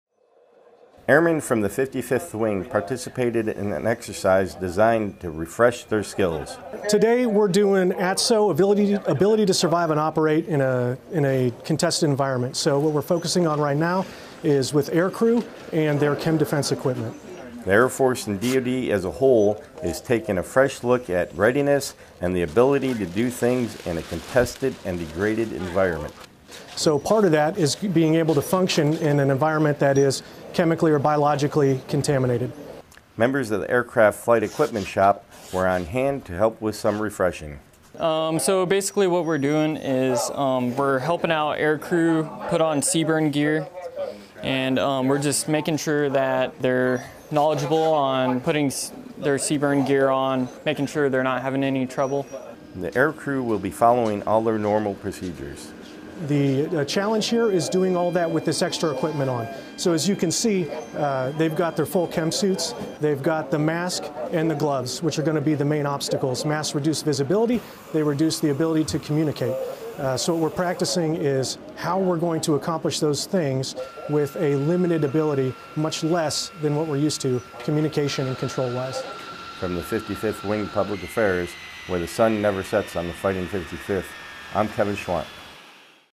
ATSO (Ability to Survive and Operate) Training
NE, UNITED STATES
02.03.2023
Video by Kevin Schwandt
55th Wing Public Affairs

Members of the 55th Wing conduct Ability to Survive and Operate (ATSO) training at Offutt AFB, Nebraska.